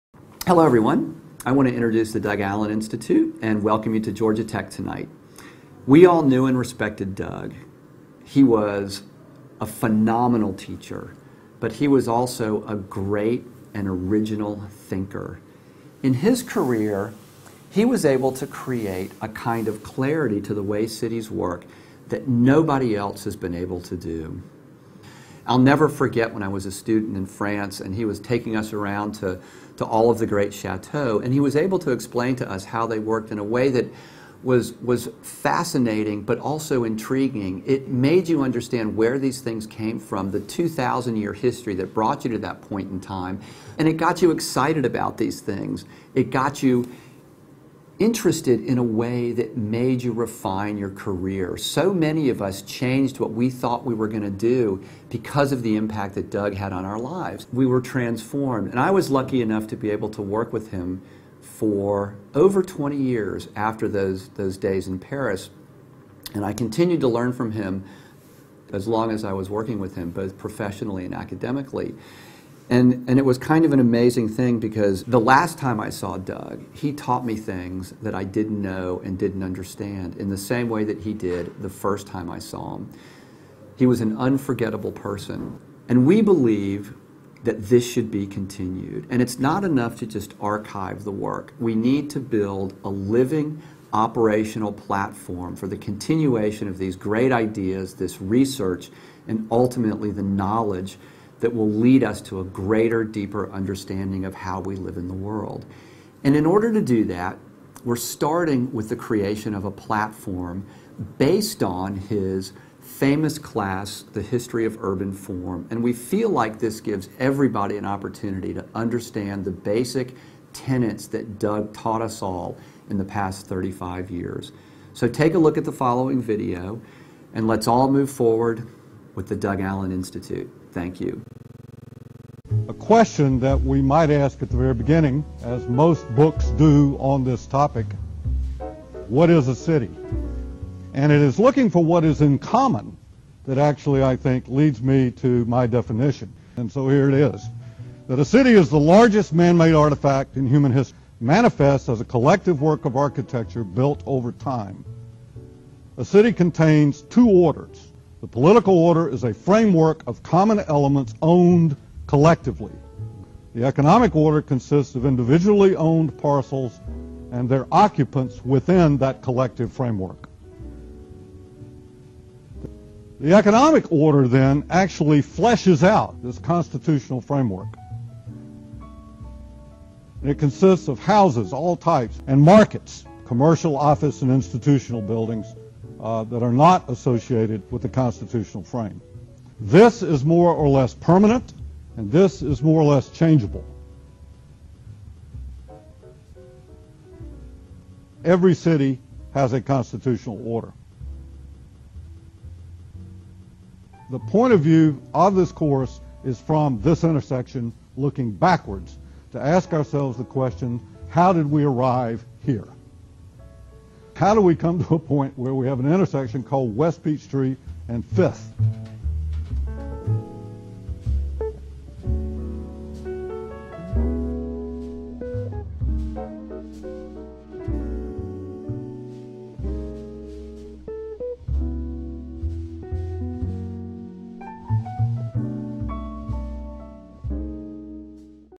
DCAI Introduction
This video was shown at the annual Douglas C. Allen lecture at Georgia Tech on November 18, 2015.  It includes a brief discussion about Doug Allen and his influence along with an introduction of our institute and a short clip from Doug's History of Urban Form course. The speaker in the video is David Green, principal at Perkins+Will and one of the DCAI's founding board members.

To learn more about the DCAI's mission and to support the refinement and release of all of Doug's History of Urban Form lectures, please

To view the entire lecture given by Susannah Drake, principal of DLANDstudio Architecture + Landscape Architecture, at Georgia Tech, please follow this